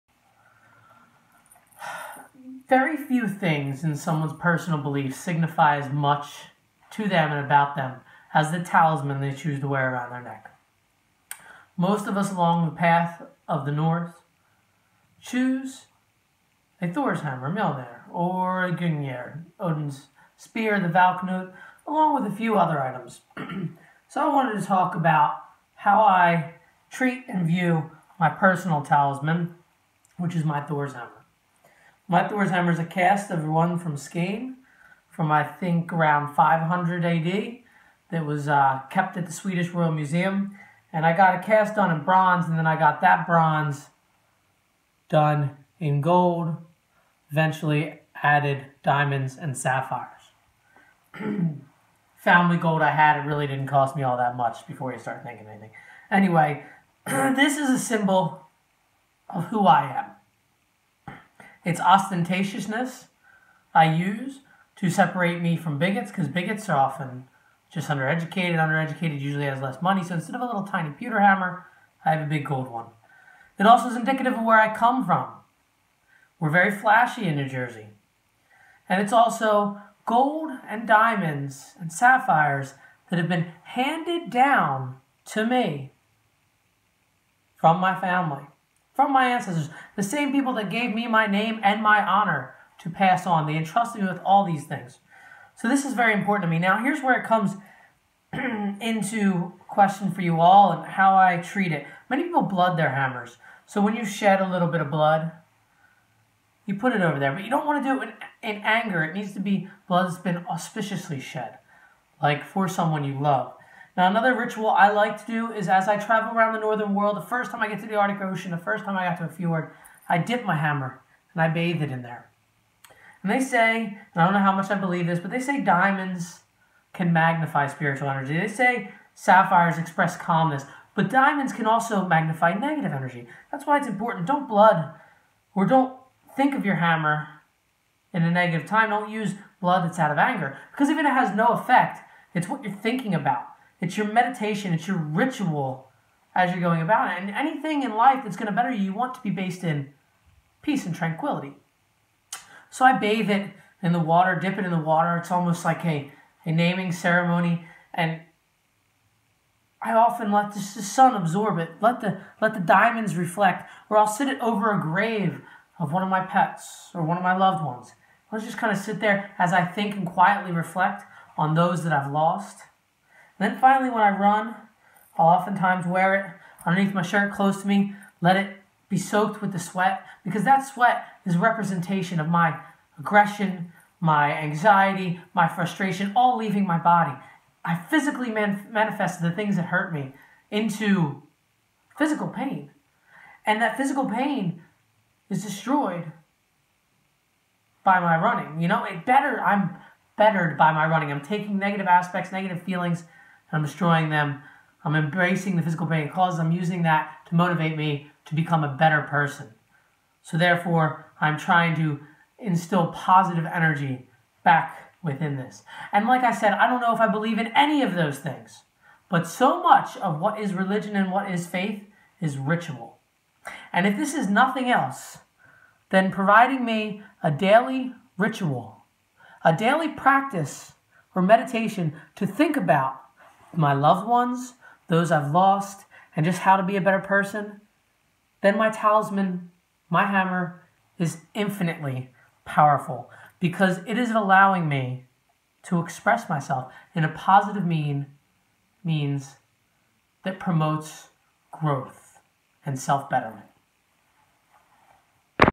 The significance of a religious Talisman
This describes how my hammer and the rituals I have created surrounding it has helped me with my daily practice of the norse traditions.